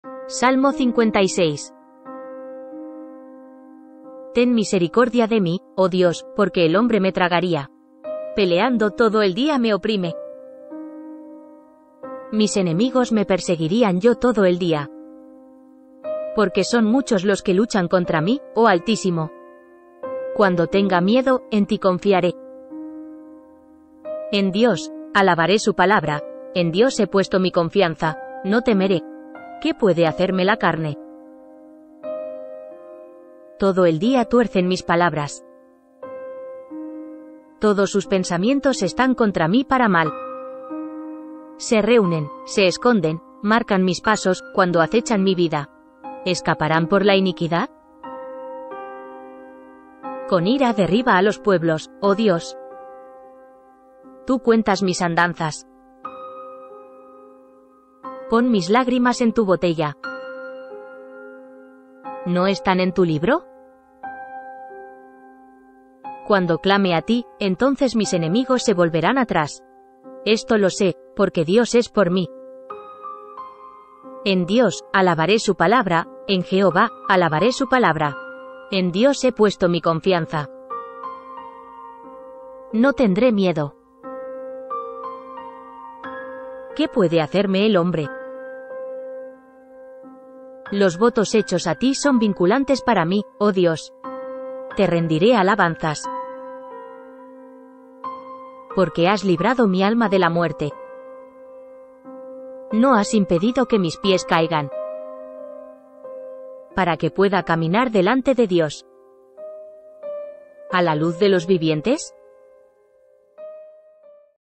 Oración efectiva para vencer tormentos
En esta poderosa oración encontrarás la paz y la fortaleza para vencer los tormentos. Una experiencia devocional que te guiará en tu camino espiritual hacia la armonía y la inspiración de la fe. psalmodia experienciadevocional armoníoespiritual inspiracióndefe viajespiritual.

Salmo 56
Oración para aliviar a los atormentadores
1 Ten misericordia de mí, oh Dios, porque el hombre me tragaría;
        Peleando todo el día me oprime.
       2 Mis enemigos me perseguirían yo todo el dia,
        Porque son muchos los que luchan contra mí, oh Altísimo.
3 Cuando tenga miedo,
        en ti confiaré.
       4 En Dios (alabaré su palabra),
        En Dios he puesto mi confianza;
        No temeré.
        ¿Qué puede hacerme la carne?
 5 Todo el día tuercen mis palabras;
        Todos sus pensamientos están contra mí para mal.
       6 Se reúnen,
        Se esconden, Marcan mis pasos,
        Cuando acechan mi vida.
       7 ¿ Escaparán por la iniquidad?
        ¡Con ira derriba a los pueblos, oh Dios!
8 Tú cuentas mis andanzas;
        Pon mis lágrimas en tu botella;
        ¿No están en tu libro?
       9 Cuando clame a ti,
        entonces mis enemigos se volverán atrás;
        Esto lo sé, porque Dios es por mí.
       10 En Dios (alabaré su palabra),
        En Jehová (alabaré su palabra),
        11 En Dios he puesto mi confianza;
        No tendré miedo.
        ¿Qué puede hacerme el hombre?
 12 Los votos hechos a Ti son vinculantes para mí, oh Dios;
        Te rendiré alabanzas,
       13 porque has librado mi alma de la muerte.
        ¿No has impedido que mis pies caigan,
        para que pueda caminar delante de Dios
        a la luz de los vivientes? Mictam de David cuando huyó de Saúl a la cueva.

Please refer to my channels:
MaGenesis @magenesis3633   - for Psalms and Bible Verses

& Magenesis1 @magenesis16445   - Old Testament , Bible Studies.